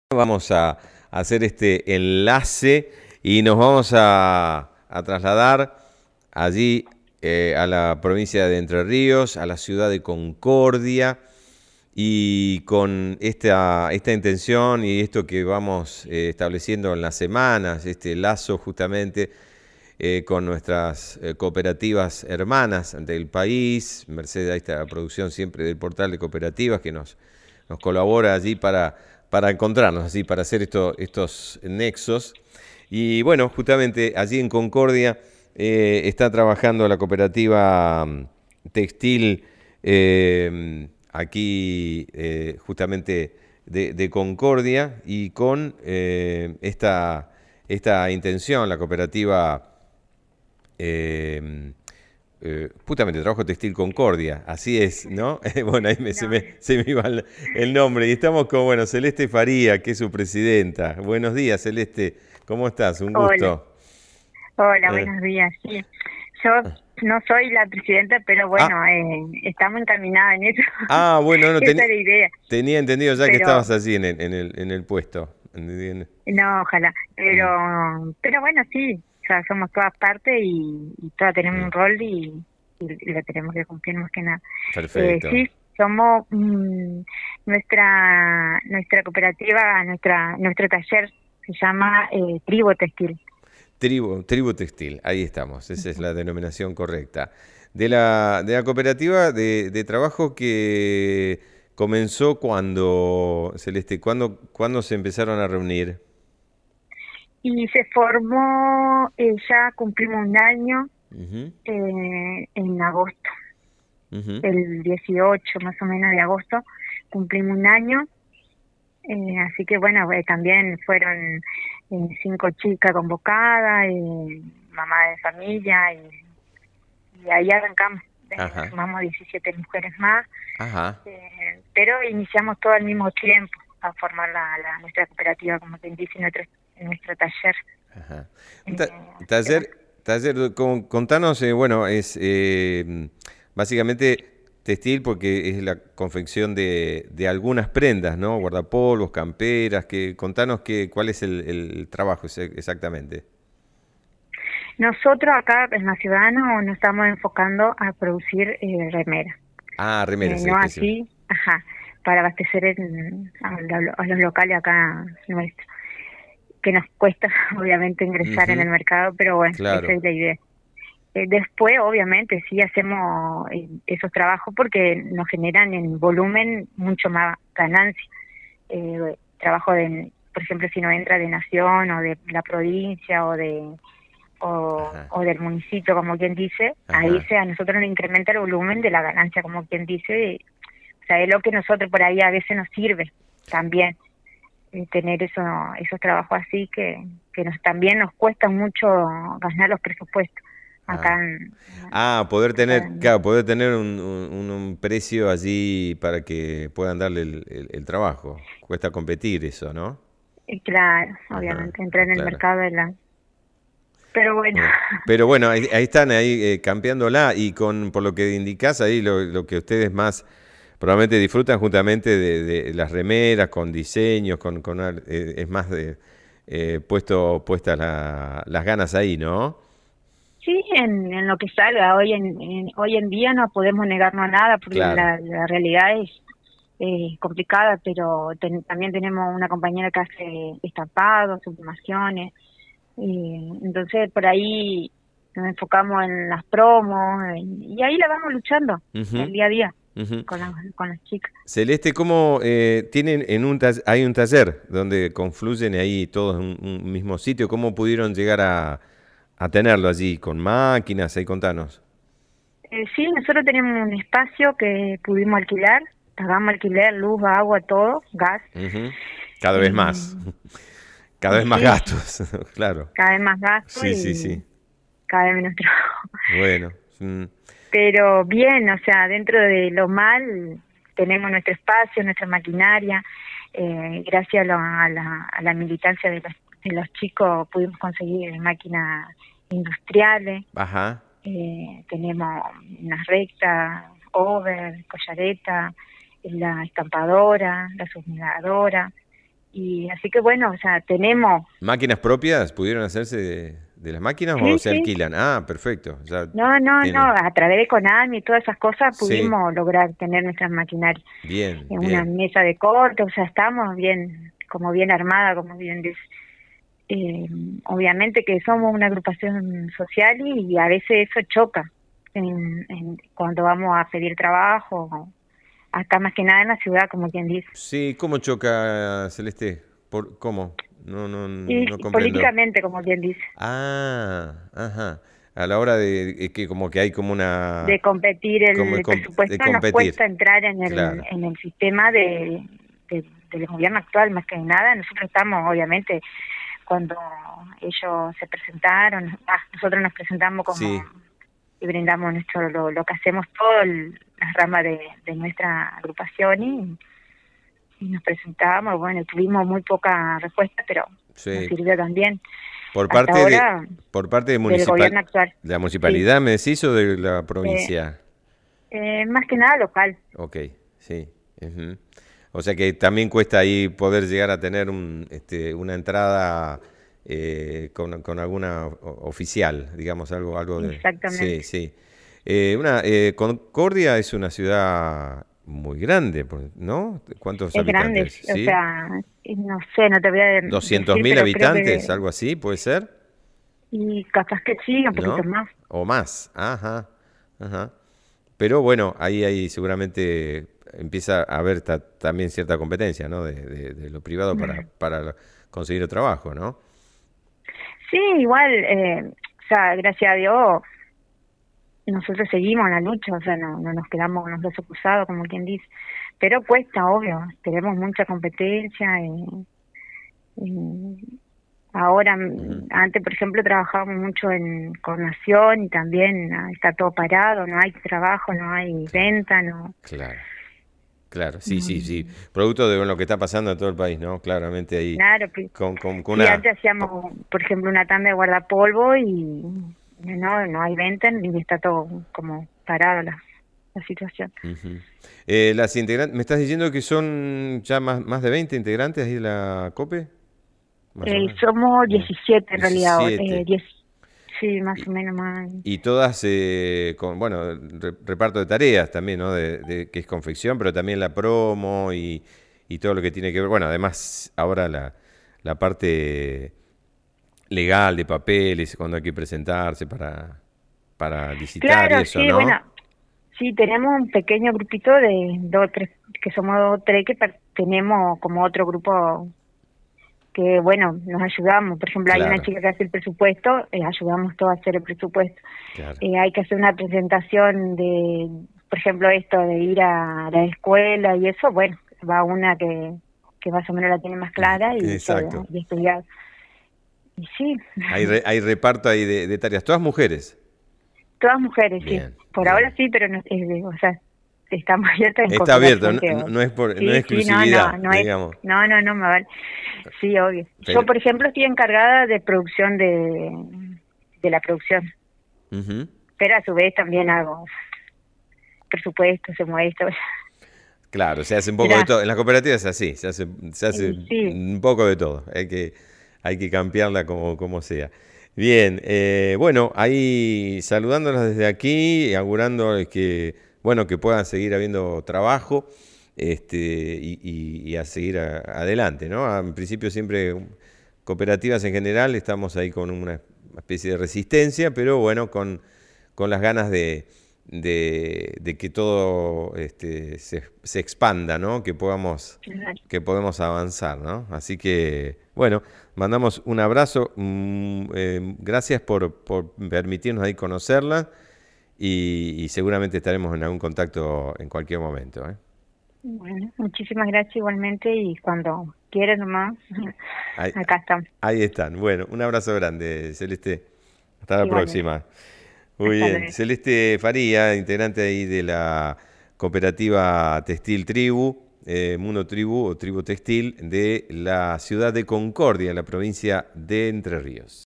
LA COOPERATIVA TEXTIL DE CONCORDIA RESISTE EL MOMENTO DIFÍCIL DE LA ECONOMÍA
En nuestro espacio semanal de contactos con grupos de la Economía Social, nos comunicamos con Celeste Faria, integrante de la Cooperativa Textil Tribu, de esa ciudad entrerriana.
Un grupo de casi 20 mujeres trabajan en un taller en la confección de vestimentas para toda la comunidad.